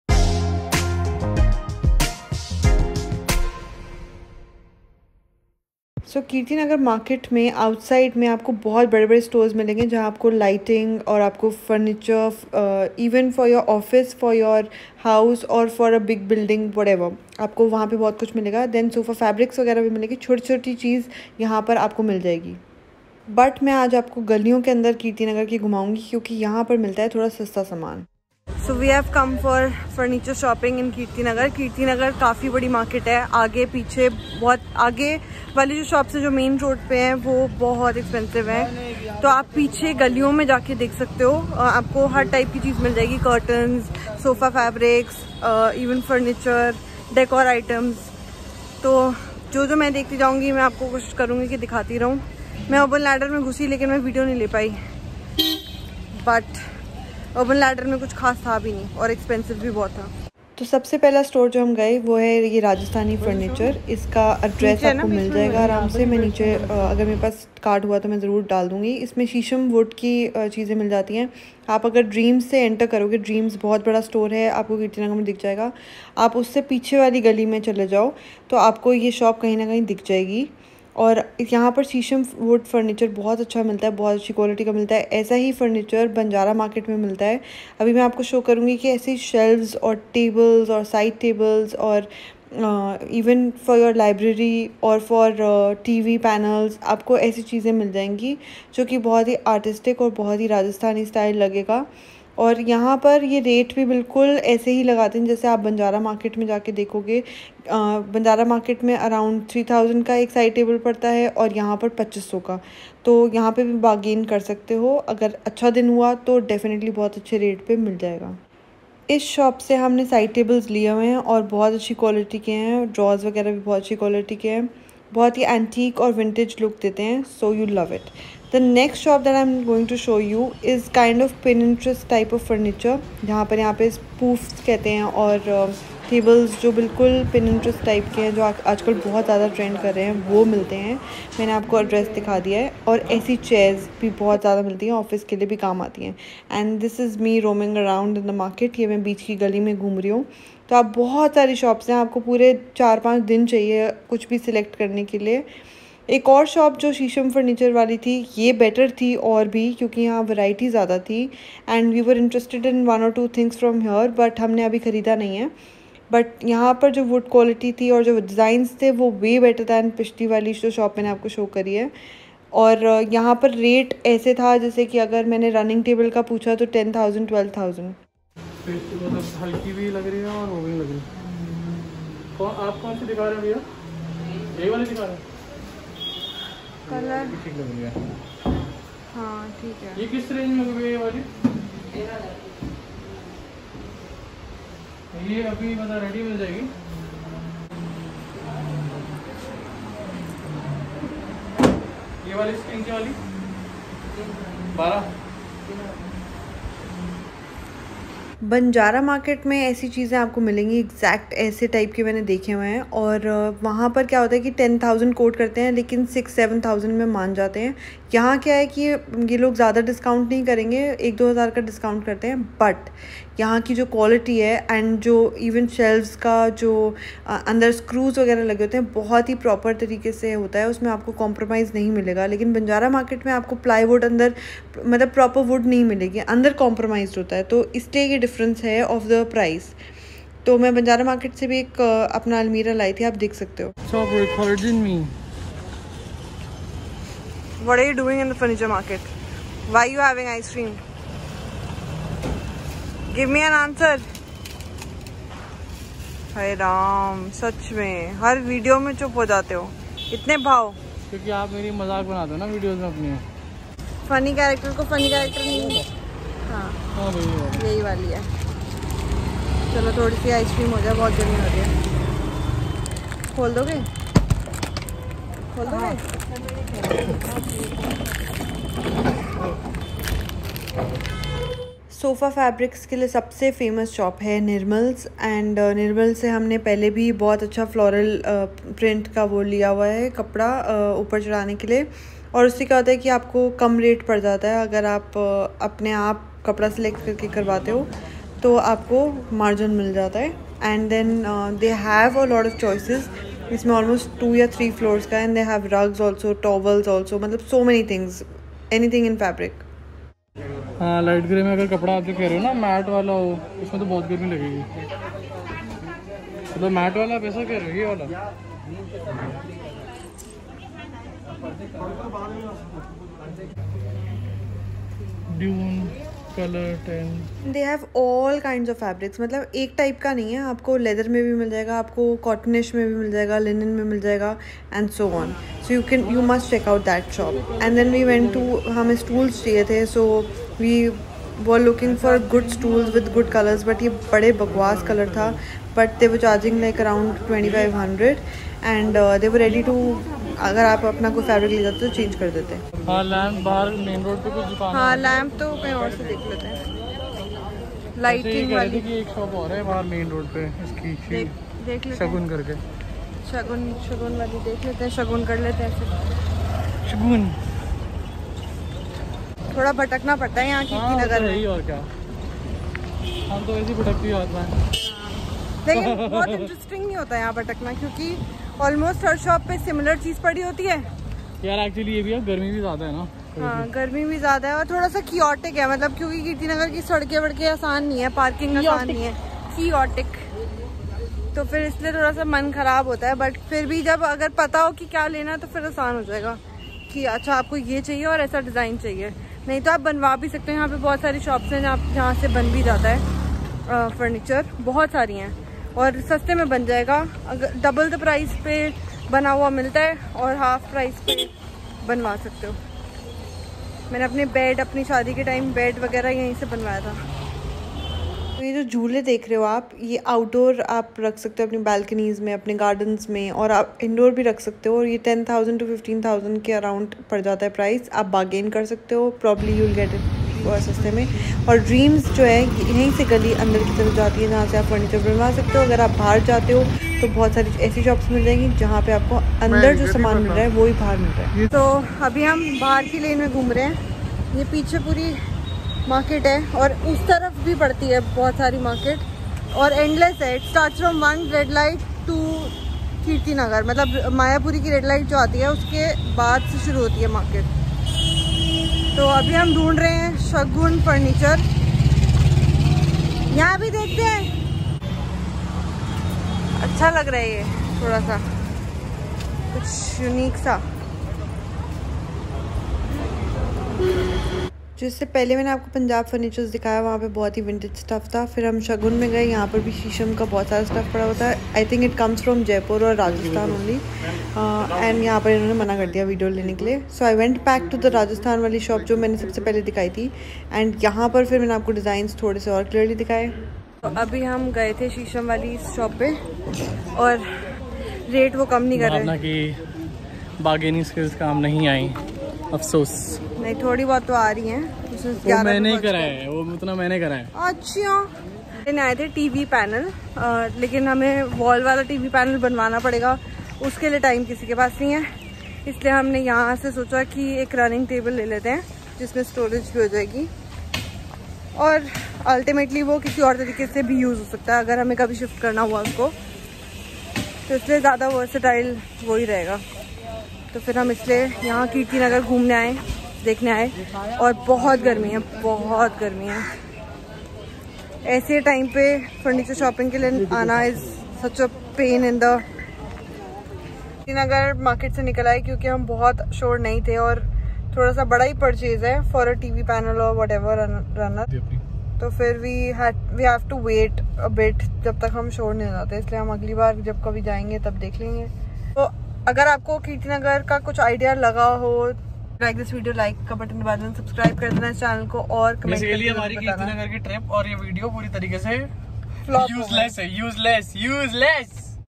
Why this furniture market in Delhi is better than Banjara Market gurgaon?
So, kirti nagar market is better only because it has long term quality of products. Banjara market has affordable but low quality. Also more classy and more variety in kirti nagar. The price difference is not too huge. Its similar if u go behind in lanes